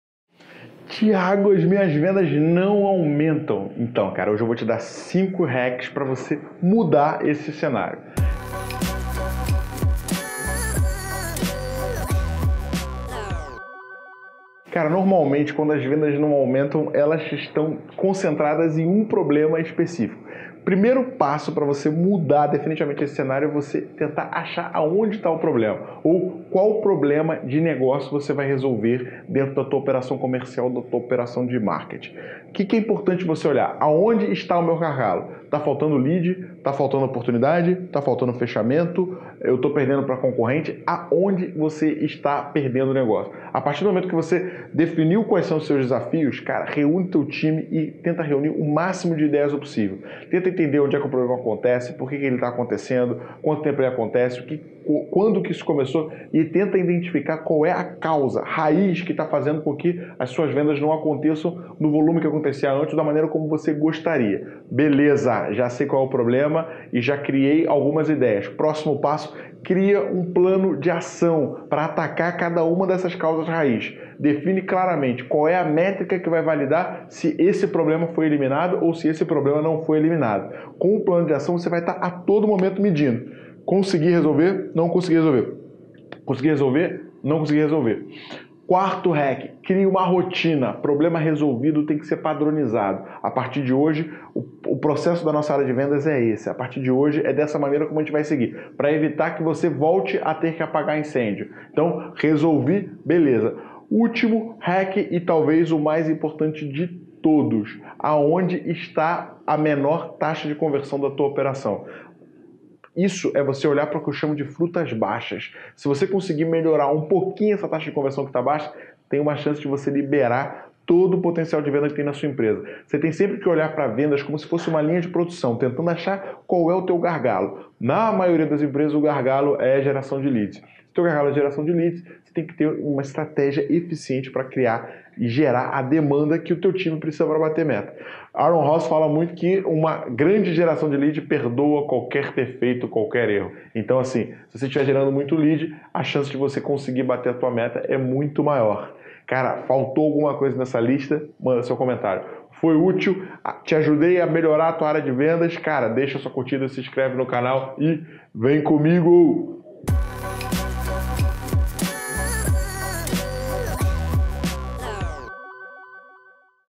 Como Melhorar suas vendas - 5 Hacks para você fazer agora - Growth Machine - Thiago Reis
Normalmente quando as vendas para exsite uma causa raiz para esse problema.
É importante observar onde está esse problema e tentar descobrir onde as coisas estão travando. Está faltando leads, está faltando oportunidades ou está faltando fechamento? Eu estou perdendo para concorrente.
A Partir do momento que você definiu seu desafio, tenta reunir seu time para pensar em ideias para resolver o problema.
Cria um plano de ação e analisa se o problema foi resolvido.
Crie uma rotina, cria um processo e podrão.
Onde está a menor taxa de conversão da sua operação. Isso é olhar as frutas baixas, onde está o gargalo, isso pode te ajudar a liberar muita receita na sua empresa.
Uma grande geração de leads perdoa qualquer erro.

Faltou alguma coisa? Manda seu comentário.